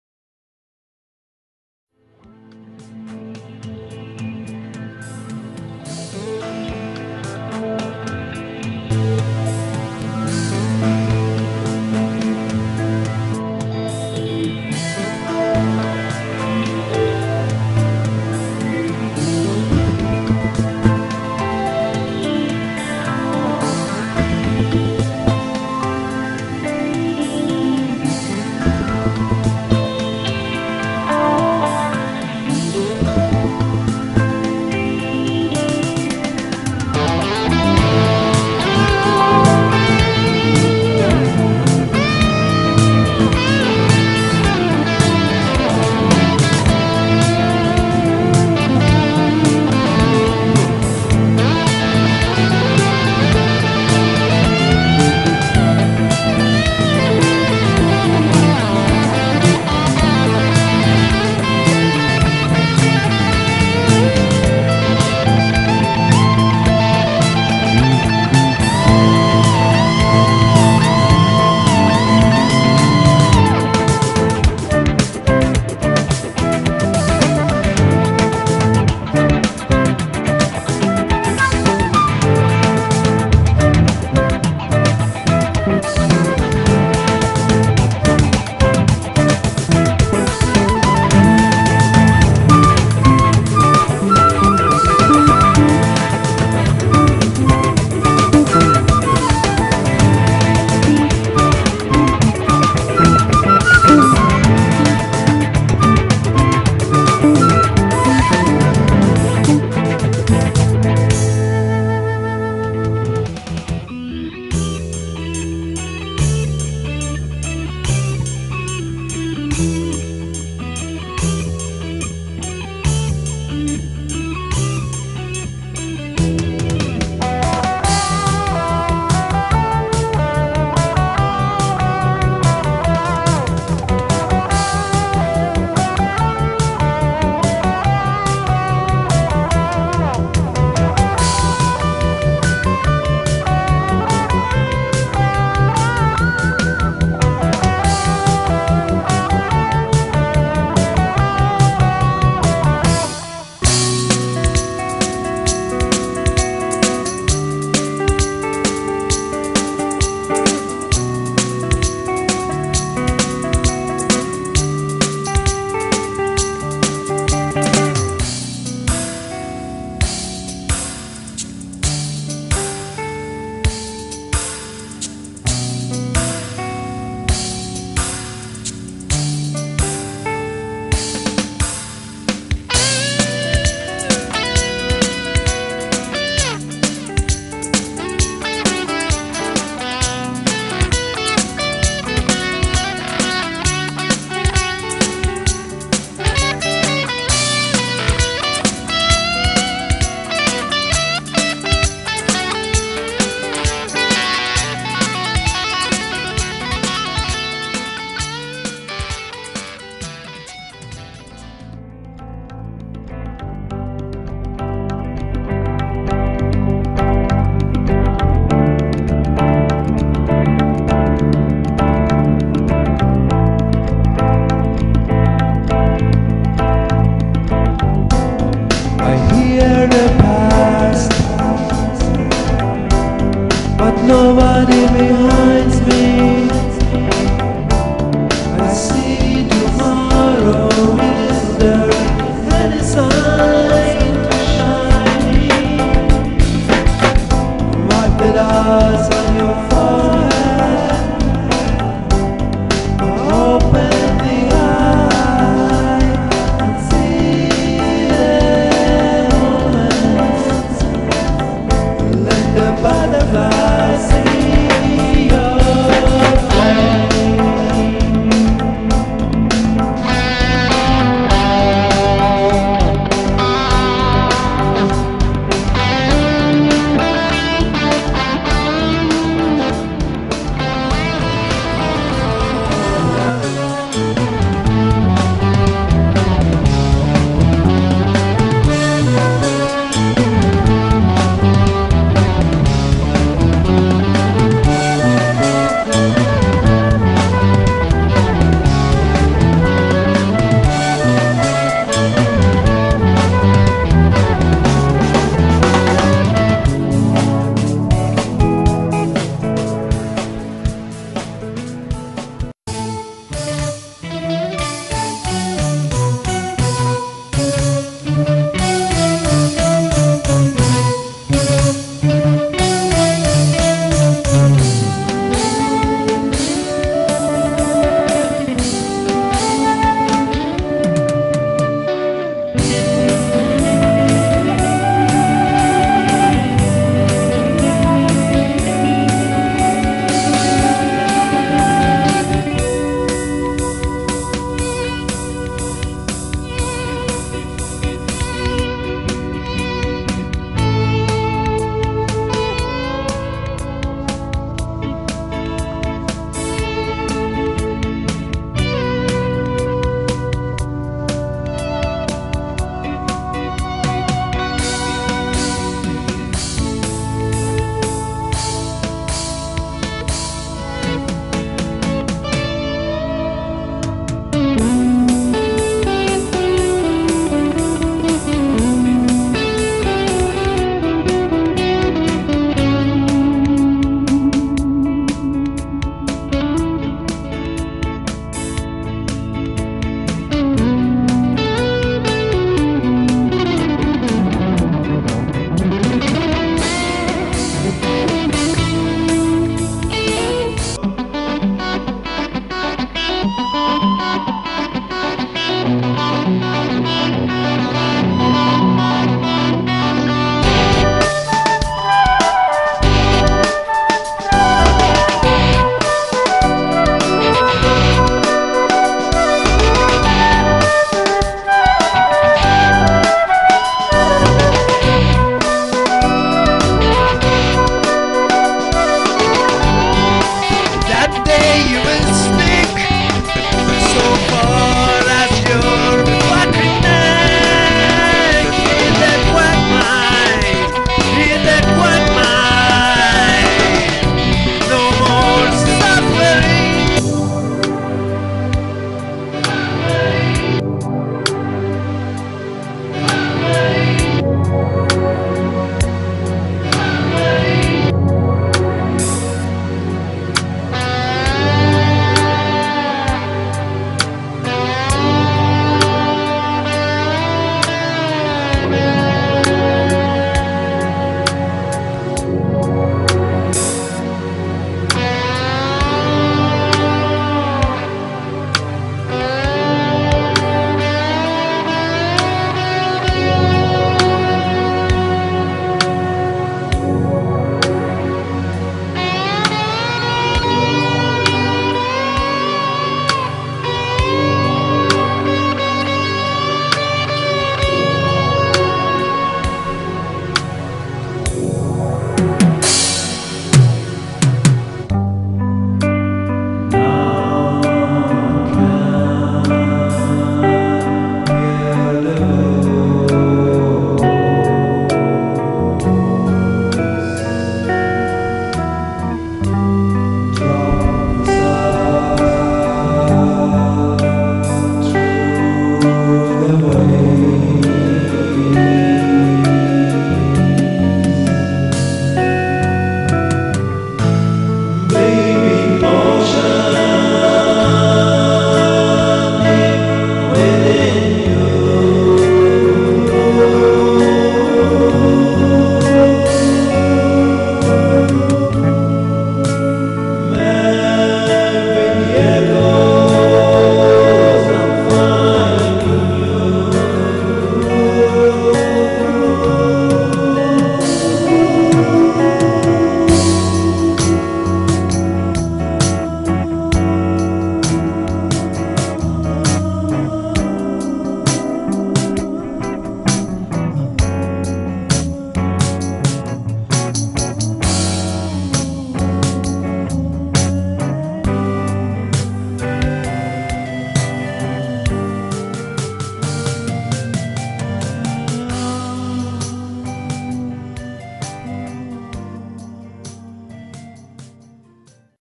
Pharos (band) - Live in Concerts ( Goa,Pune&.. 2004-2010)
Pharos ( Persian progressive rock band) formed in 2001 in Tehran.